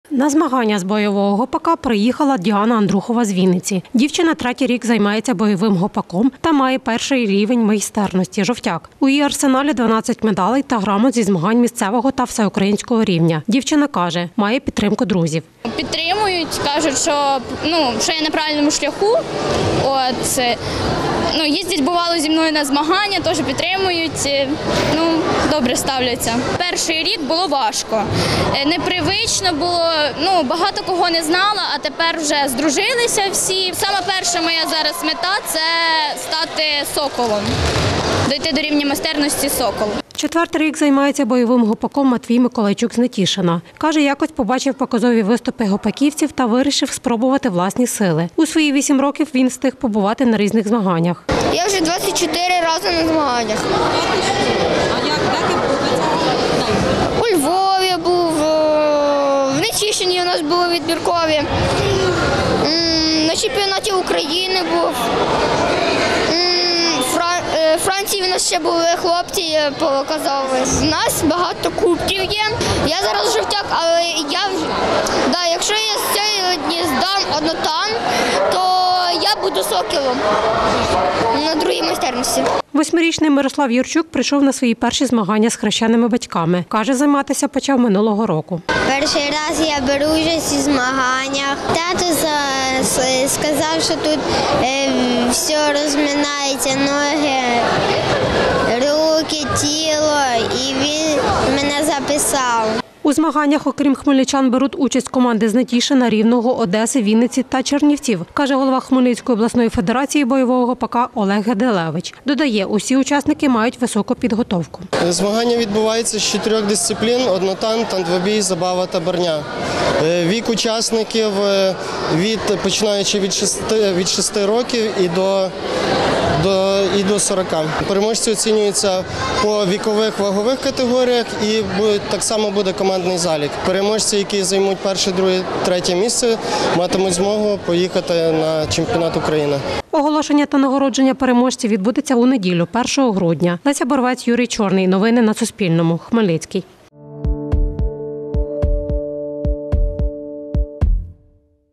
150 учасників беруть участь у четвертих змаганнях за кубок Хмельницької області з бойового гопака
150 учасників з Хмельницького та України беруть участь у четвертих змаганнях за кубок Хмельницької області з бойового гопака, які стартували сьогодні в обласному центрі. Про це розповів голова Хмельницької обласної федерації бойового гопака Олег Геделевич.